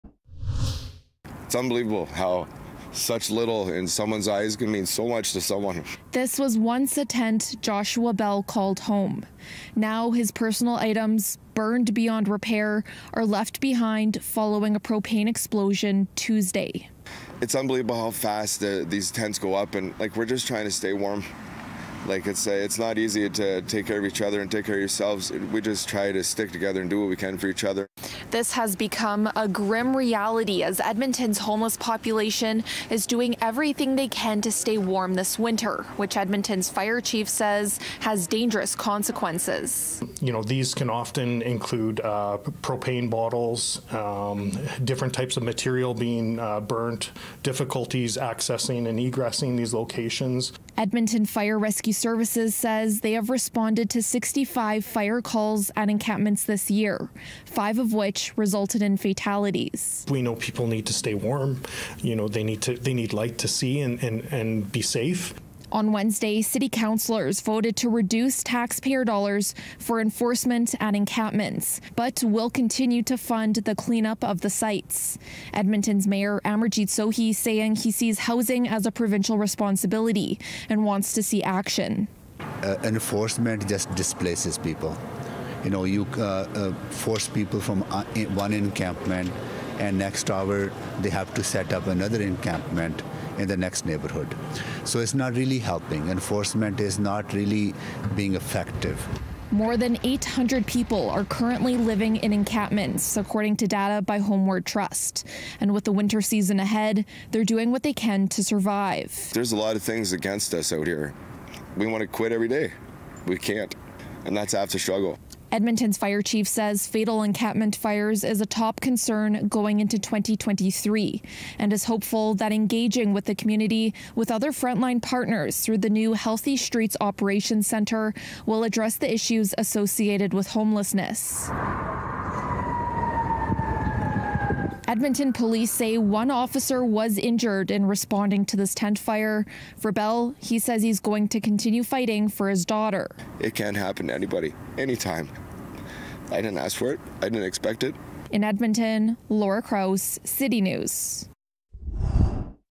Edmonton’s fire chief says encampment fire are a top concern
Edmonton’s fire chief says encampment fires are a top priority, saying crews have responded to 65 calls this year. Laura Krause speaks to a man experiencing homelessness whose tent burned down this week.